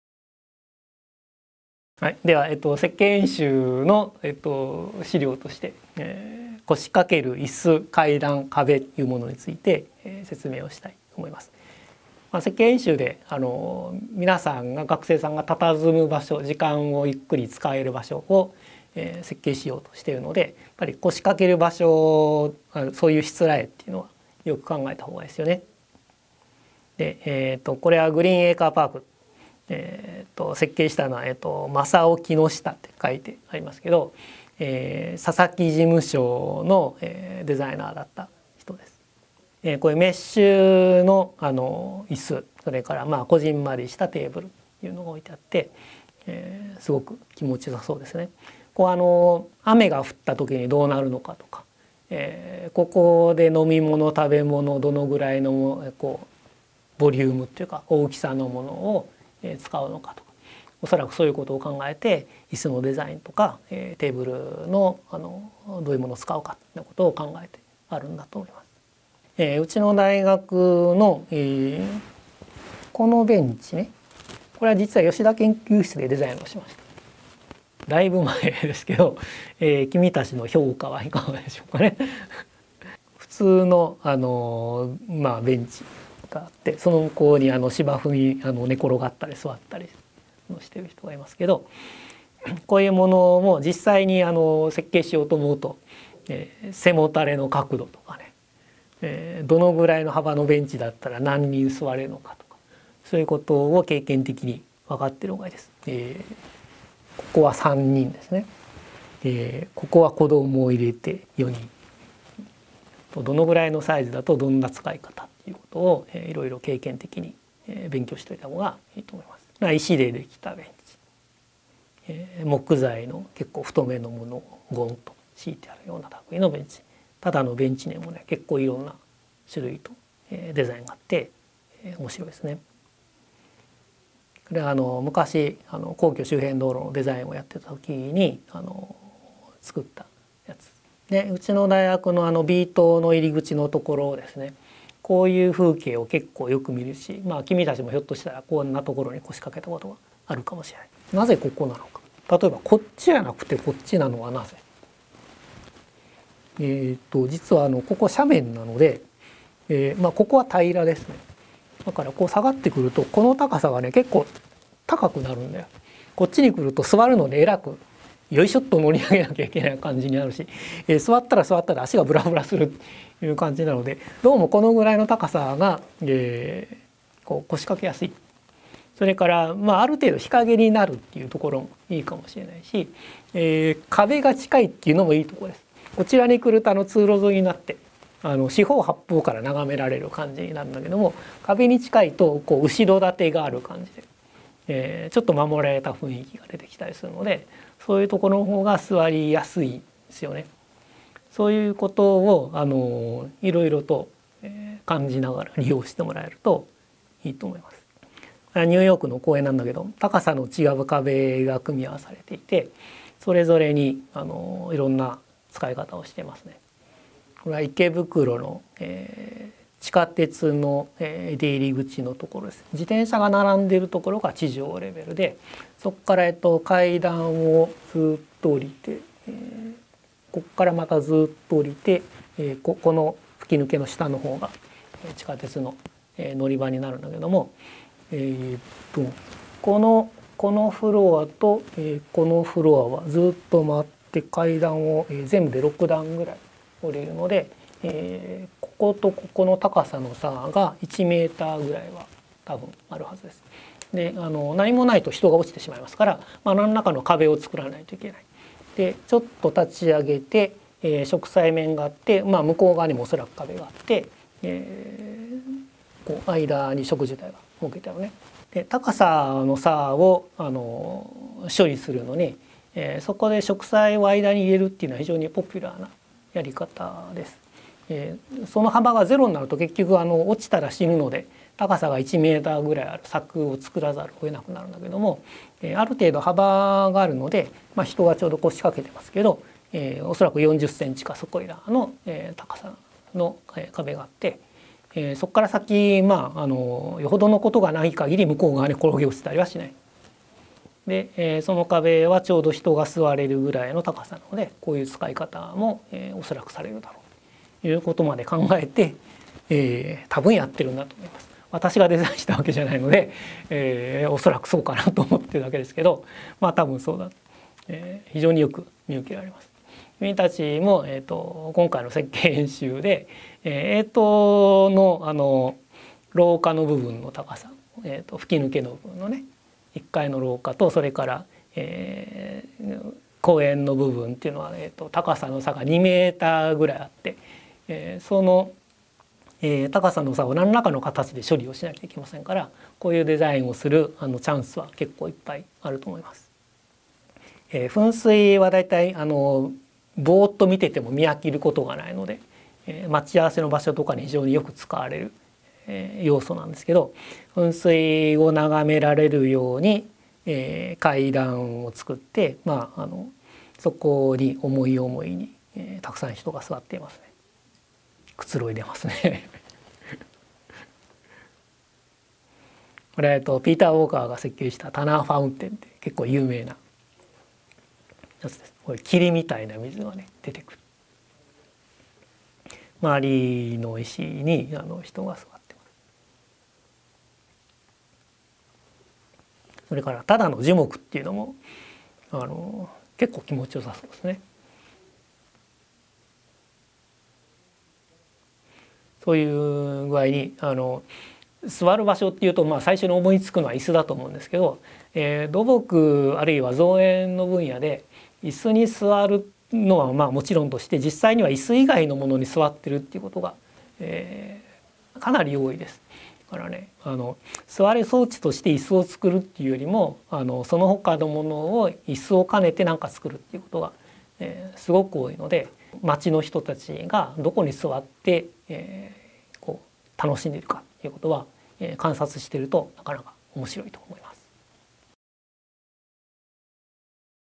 腰掛ける椅子、階段、壁
高知工科大学システム工学群　建築・都市デザイン専攻の講義「景観デザイン」の教材です。